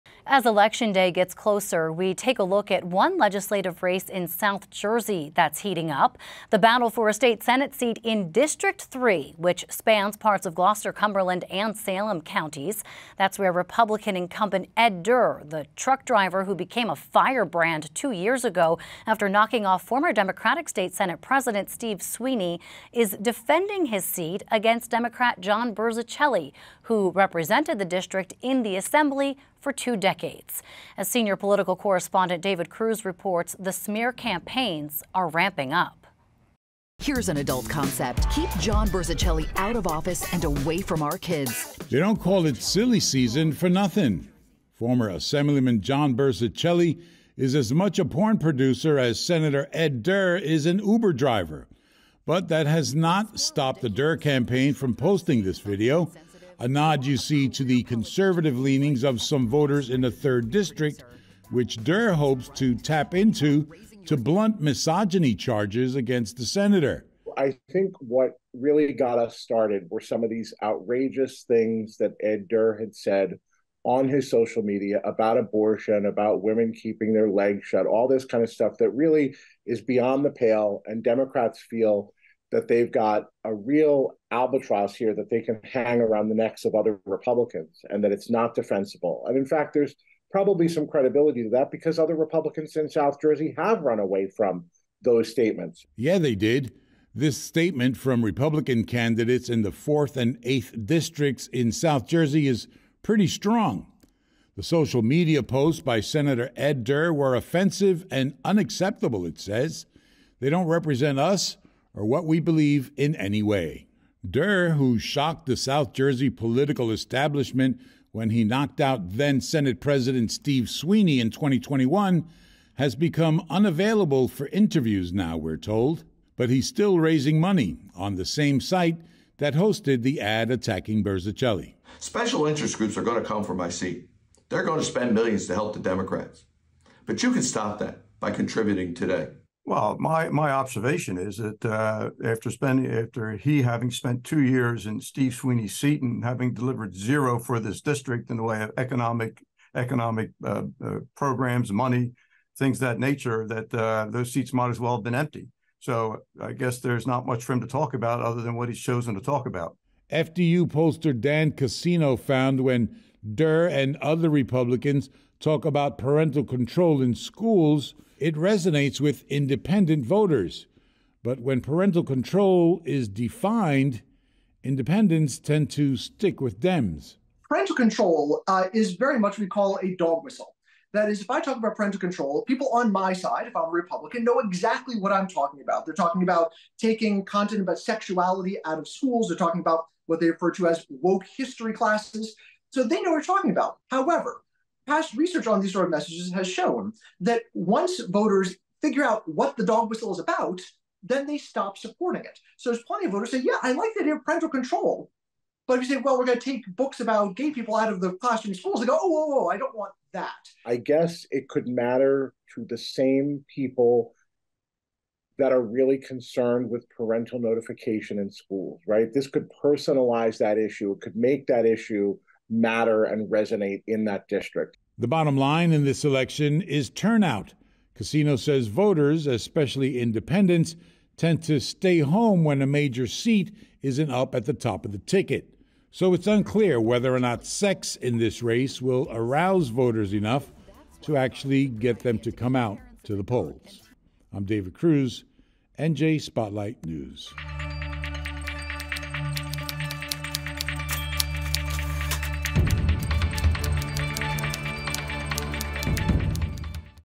Durr draws criticism for attacks on Democratic challenger
As Election Day draws closer, the Senate race in the 3rd Legislative District is heating up. Republican incumbent Ed Durr, who defeated Democratic Senate President Steve Sweeney in 2021, is defending his seat against Democrat John Burzichelli, who represented the district in the Assembly for two decades.